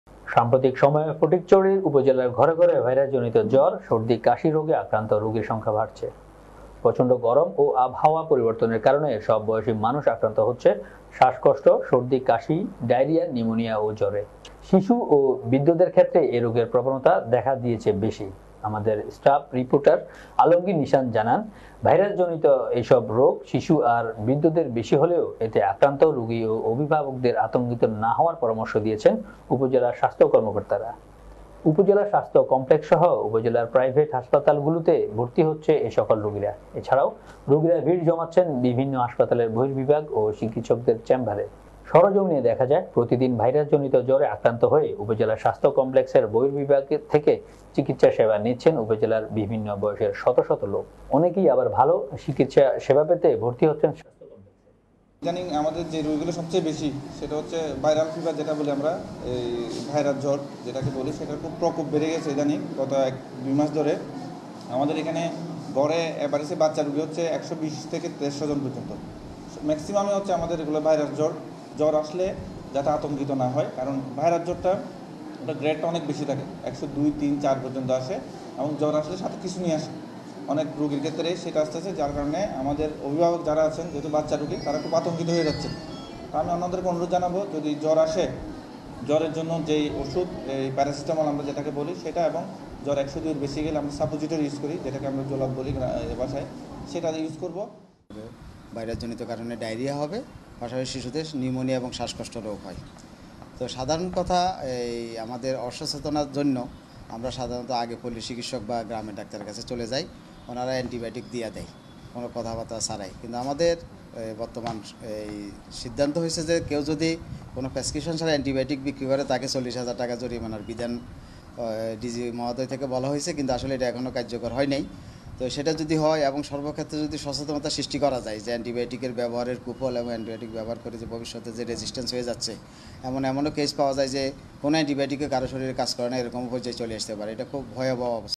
ফটিকছড়িতে ঘরে ঘরে জ্বরের প্রকোপ বৃদ্ধি, আতংকিত না হওয়ার পরামর্শ চিকিৎসদের
সাম্প্রতিক সময়ে ফটিকছড়ি উপজেলার ঘরে ঘরে ভাইরাসজনিত জ্বর, সর্দি, কাশি রোগে আক্রান্ত রোগীর সংখ্যা ভাড়ছে। পচন্ড গরম ও আবহাওয়া পরিবর্তনের কারণে সব বয়সি মানুষ আক্রান্ত হচ্ছে শ্বাসকষ্ট, সর্দি, কাশি, ডায়রিয়া, নিউমনিয়া ও জ্বরে। তবে শিশু ও বৃদ্ধদের ক্ষেত্রে এ রোগের প্রবণতা দেখা দিয়েছে বেশি